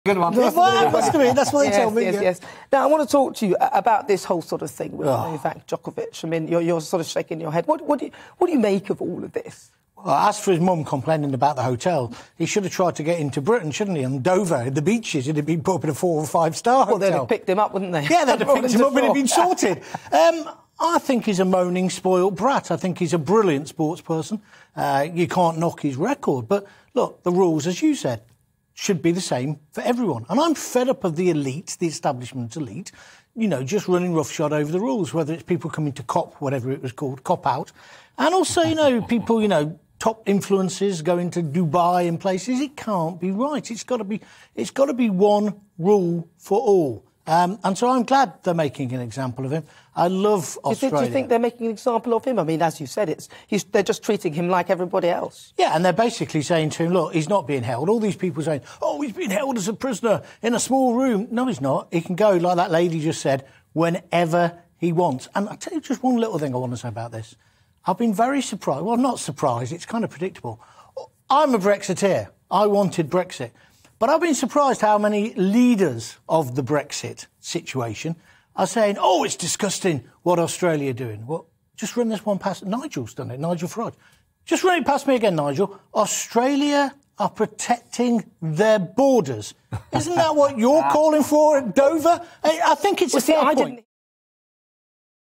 Djokovic is a moaning spoilt brat. The vaccine rules should be the same for everyone | Jon Gaunt
Broadcaster Jon Gaunt on the world number one being denied entry to Australia by border authorities following vaccine controversy.